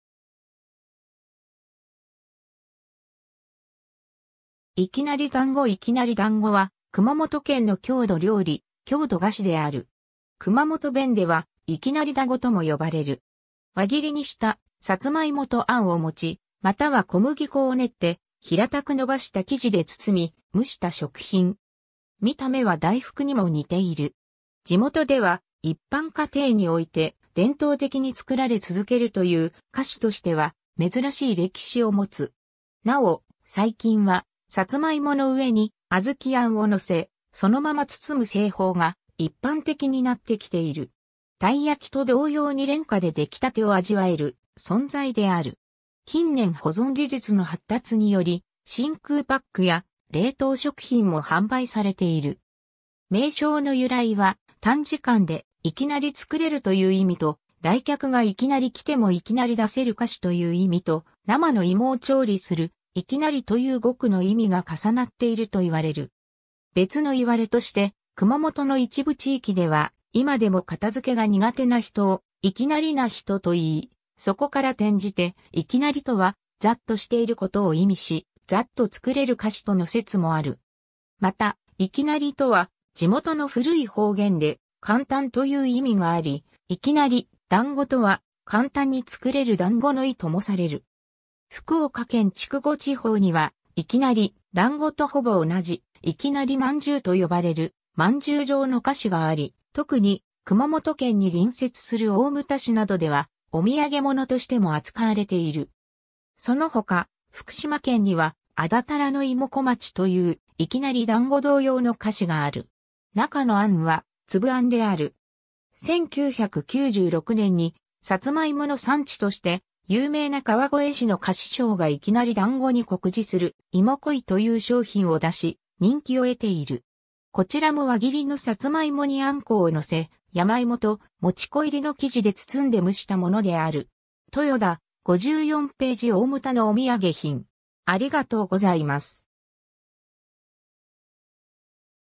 いきなり団子
いきなり団子 いきなり団子（いきなりだんご）は、熊本県の郷土料理、郷土菓子である。
 熊本弁では「いきなりだご」とも呼ばれる。
 輪切りにしたサツマイモと餡（小豆あん）を餅（ねりもち）、または小麦粉を練って平たく伸ばした生地で包み、蒸した食品。
見た目は大福にも似ている。
 地元では一般家庭において伝統的に作られ続けるという、菓子としては珍しい歴史を持つ。
なお、最近はサツマイモの上にアズキ餡（こしあん、またはつぶあん）を乗せ、そのまま包む製法が一般的になってきている。
 たい焼きと同様に廉価で出来立てを味わえる存在である。
近年保存技術の発達により、真空パックや冷凍食品も販売されている。
 名称の由来は短時間で「いきなり」作れるという意味と、来客がいきなり来てもいきなり出せる菓子という意味と、生の芋を調理する「生き成り」（いきなり）という語句の意味が重なっていると言われる。
 別の言われとして、熊本の一部地域では今でも片付けが苦手な人を「いきなりな人」と言い、そこから転じて「いきなり」とは「ざっとしている」ことを意味し、ざっと作れる菓子との説もある。
また、「いきなり」とは地元の古い方言で「簡単」という意味があり、「いきなり団子」とは「簡単に作れる団子」の意ともされる。
 福岡県筑後地方には、いきなり団子とほぼ同じ「いきなり饅頭」と呼ばれる饅頭状の菓子があり、特に熊本県に隣接する大牟田市などではお土産物としても扱われている。
そのほか、福島県には「あだたらのいも小町」といういきなり団子同様の菓子がある。
中のあんは、つぶあんである。
 1996年に、さつまいもの産地として有名な川越市の菓子商がいきなり団子に酷似する「いも恋」という商品を出し、人気を得ている。
こちらも輪切りのさつまいもにあんこを乗せ、山芋ともち粉入りの生地で包んで蒸したものである。
 ^ 豊田（2006）：54ページ ^ 大牟田のお土産品（大牟田観光協会ホームページ内）